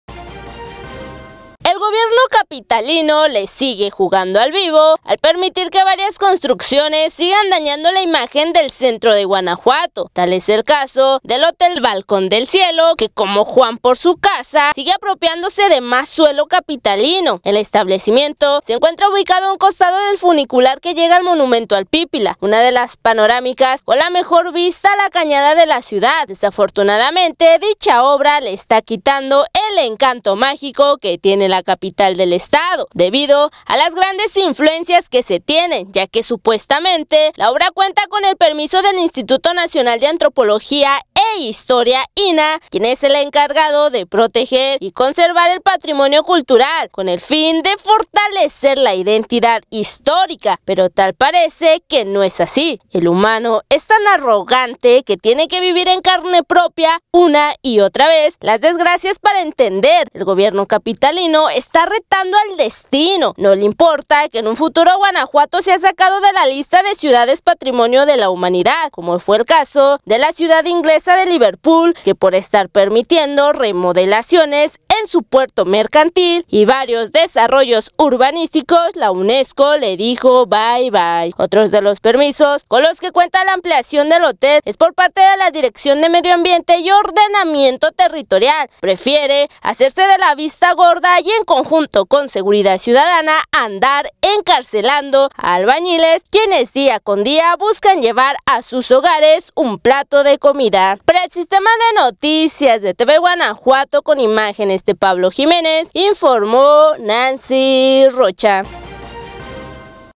Hotel Balcón del Cielo sigue apropiándose de más suelo capitalino con permiso del Gobierno.
Como Juan por su casa, el 🏨 hotel Balcón del Cielo sigue apropiándose de más suelo capitalino con permiso del gobierno, quien sigue jugando al vivo al permitir que construcciones dañen la imagen del centro histórico.❌